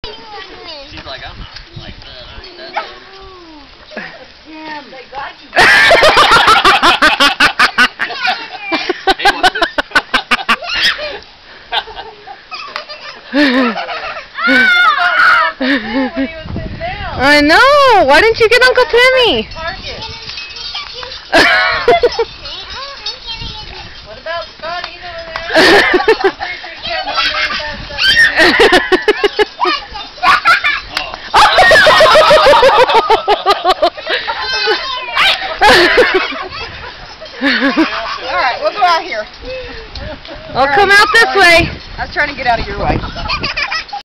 Alexis & Azayle Playing with Cascarones
Alexis & Azayle throwing and smashing cascarones (egg shells w/confetti inside) out at my Granny's house.

I apologize for my annoying and loud laughing! hahaha